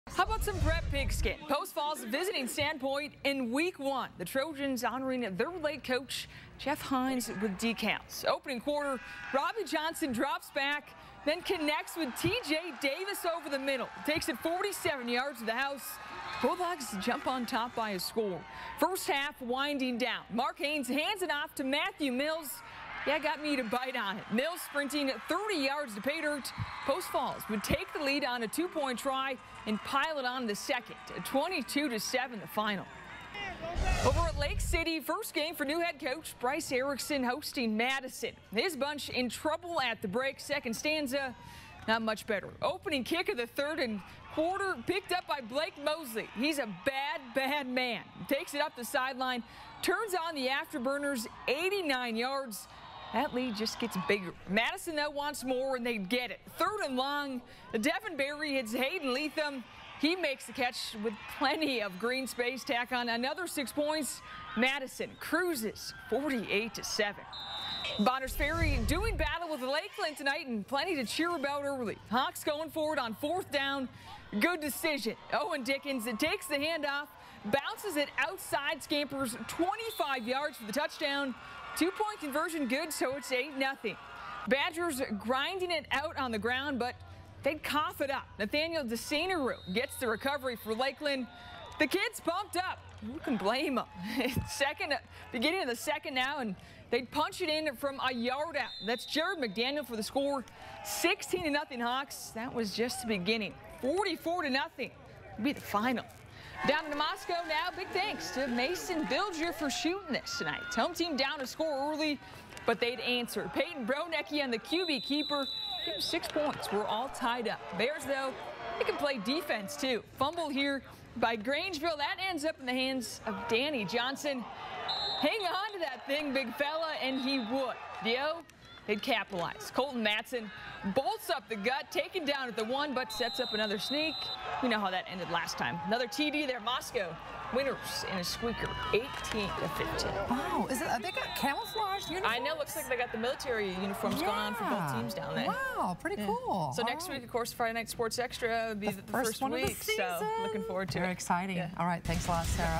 Post Falls honors late coach, Jeff Hinz, as football season kicks off
The 2016 prep football season kicked off throughout Idaho on Friday night highlighted by Post Falls beating Sandpoint in its first game without head coach Jeff Hinz at the helm.